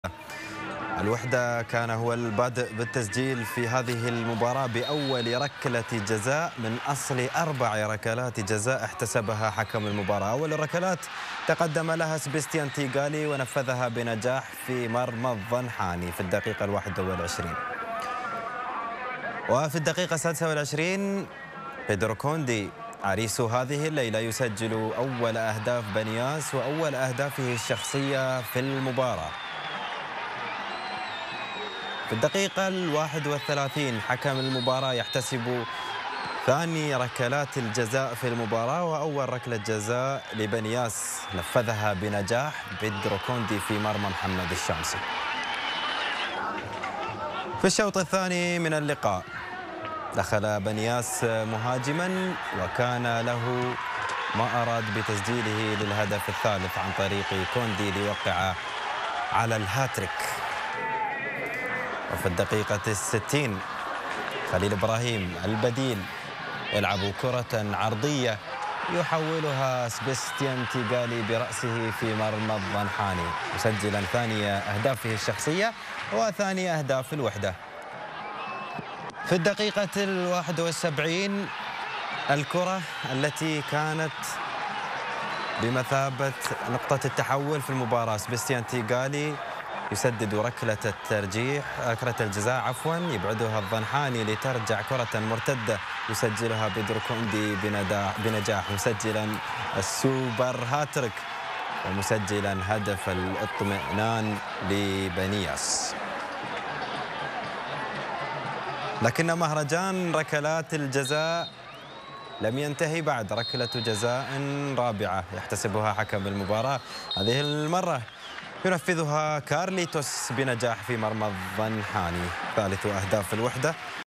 ملخص مباراة بني ياس والوحدة 4-3 | أجمل مباراة | دوري الخليج العربي
الجولة الثالثة من دوري الخليج العربي